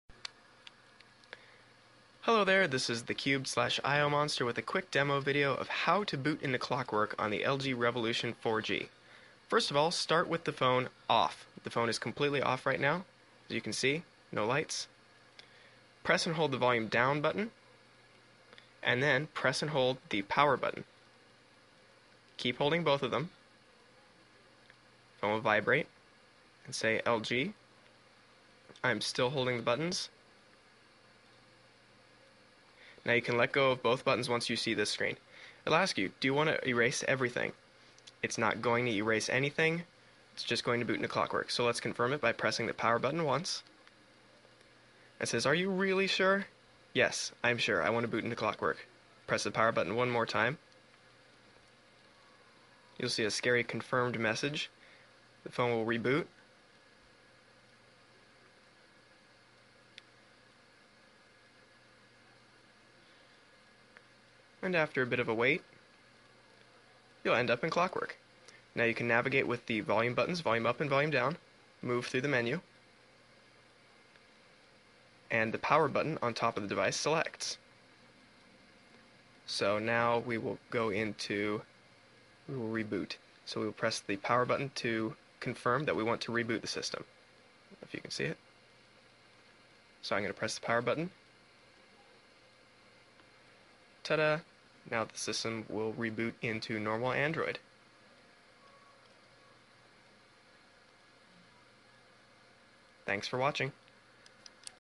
LG Revolution 4G Boot into ClockworkMod Recovery
How to enter ClockworkMod Recovery from the device being powered off.